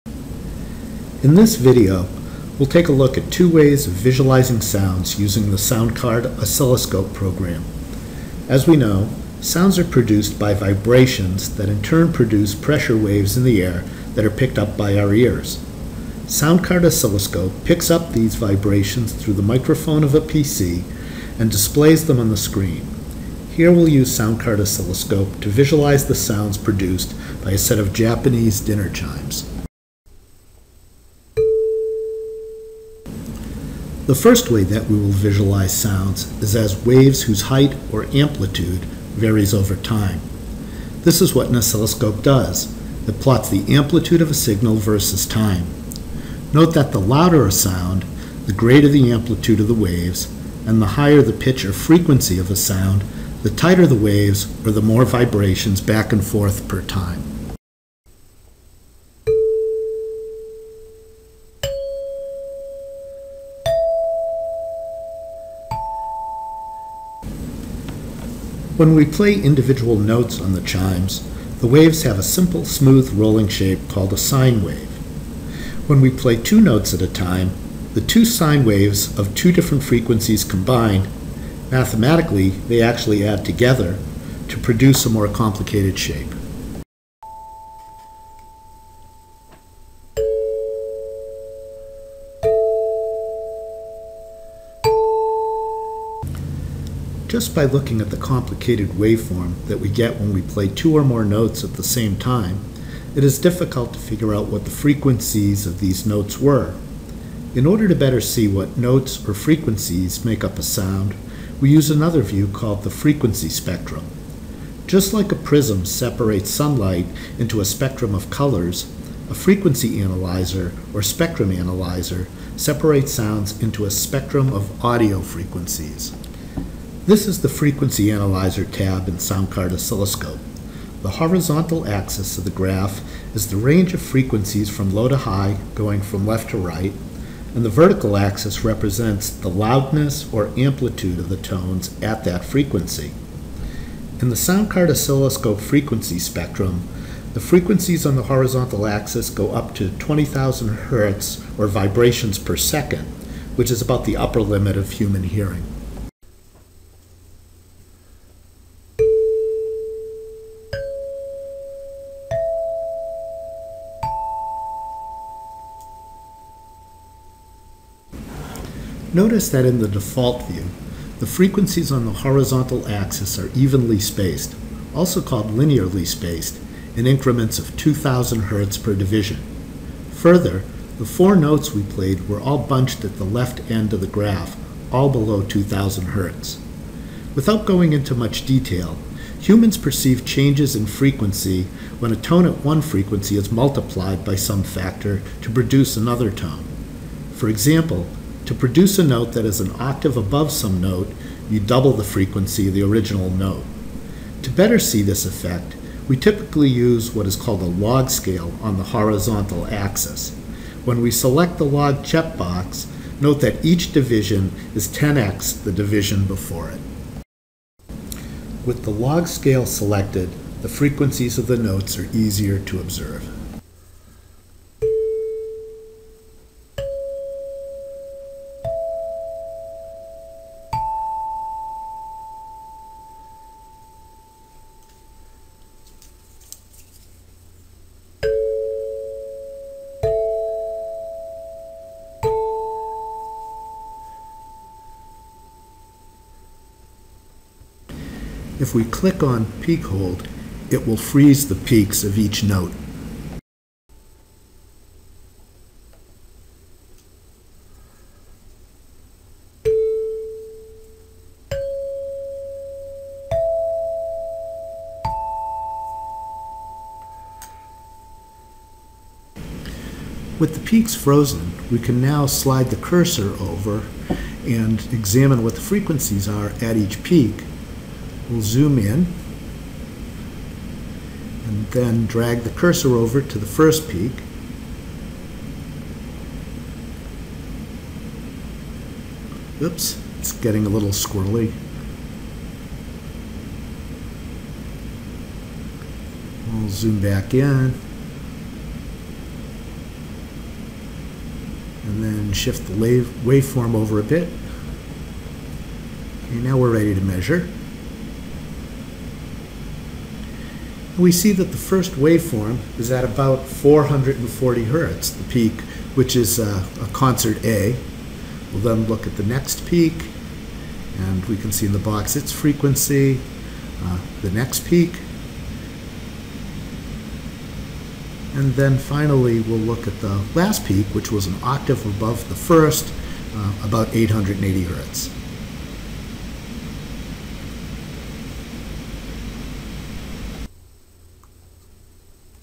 Visualizing Musical Sounds with Soundcard Oscilloscope
Tutorial on using Soundcard Oscilloscope to visualize musical sounds from a set of Japanese dinner chimes.  This tutorial was prepared for the University of Notre Dame WAVES project, which is developing STEAM (STEM + Arts) educational programs.